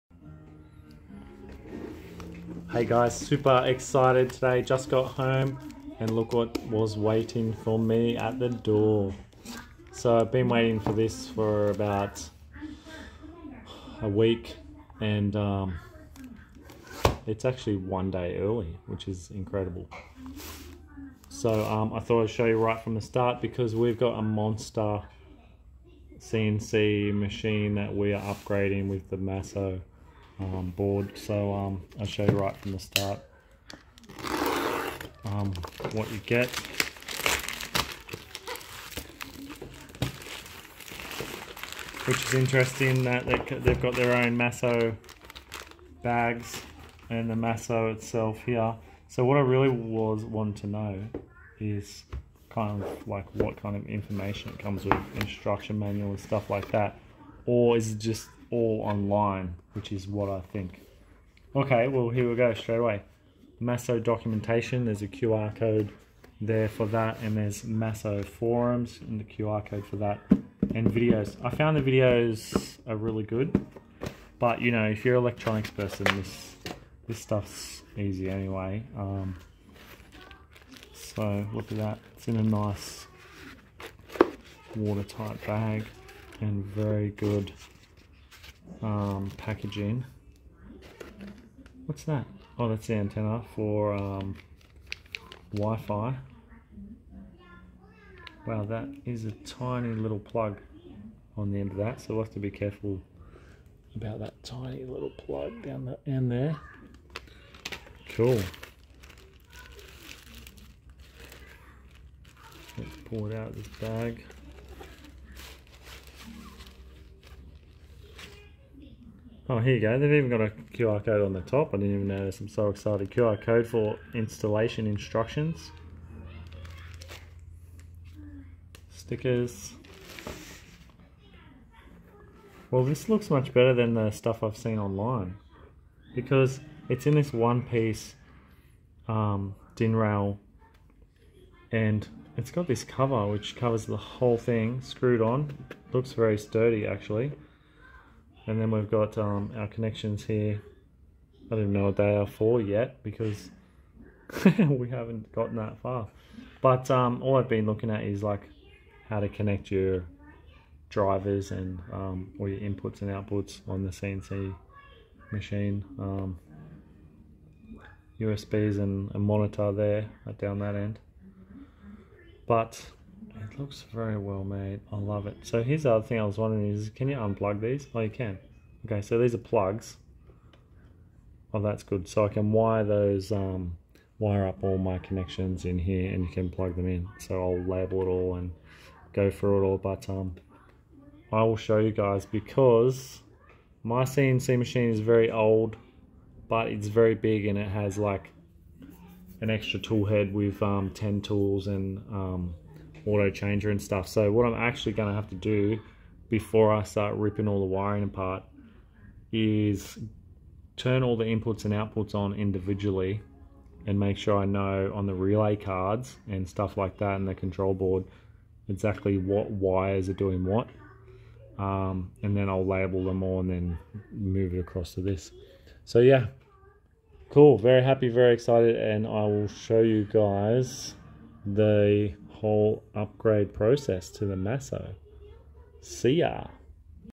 MASSO CNC CONTROLLER ARRIVED!
First look at the MASSO G3 ready for my new big project CNC machine upgrade.